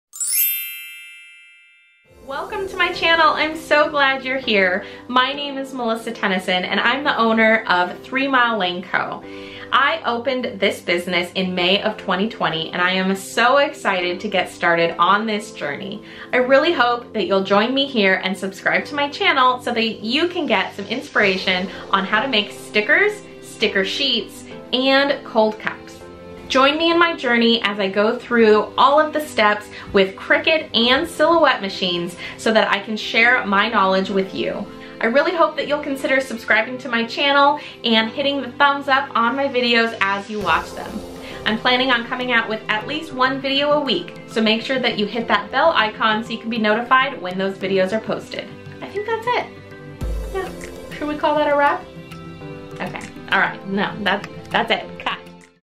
Channel Introduction | 3milelaneco Tutorials
Welcome to my tutorials channel! I'll be diving in depth on topics from sticker making to vinyl crafts to being a small business owner. I'm so glad to have you here!